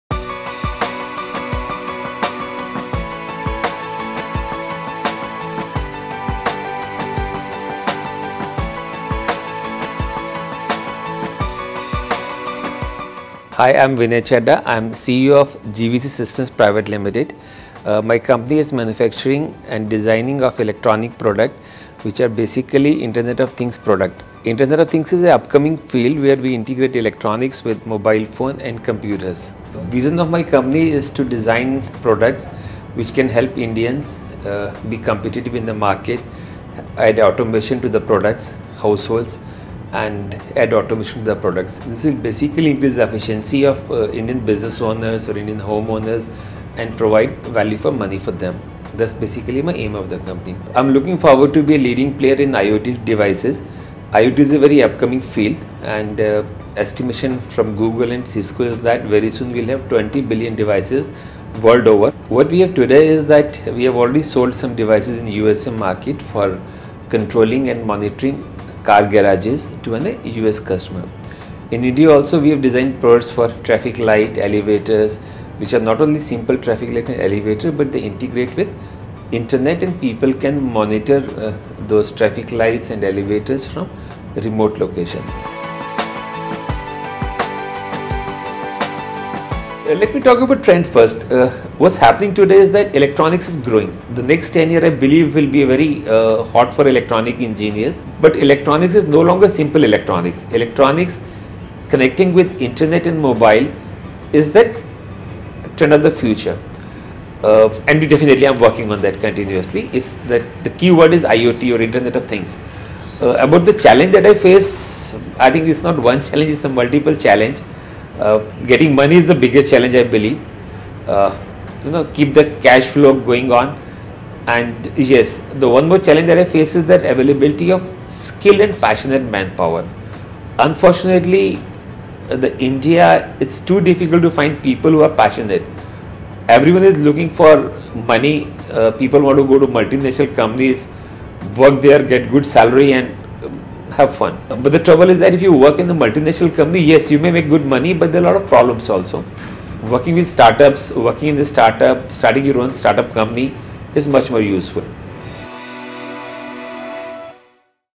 SME Heroes: Vinay Chadda, CEO of GVC Systems Pvt Ltd
GVC Systems is engaged in manufacturing and designing of electronic products. The company was honored with the "Spirit of Manufacturing" award for excellence in innovation and IT adoption.